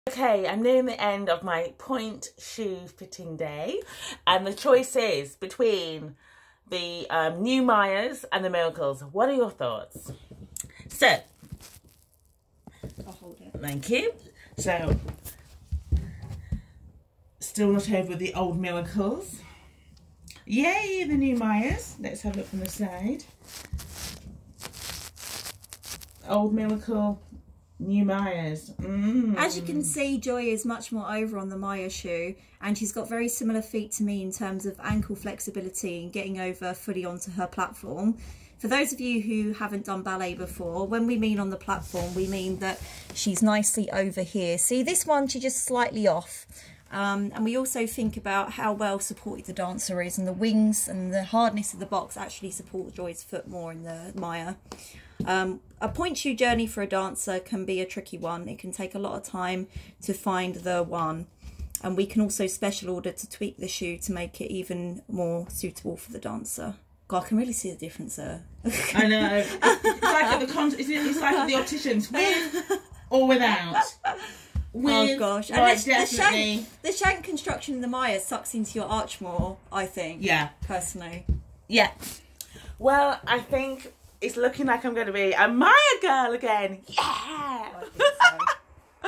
Straight to the pointe fits Joy carter
Pointe shoe fitting day , trying Grisko  Russian pointe shoes, with expert Leanne bisson from company straight to the pointe..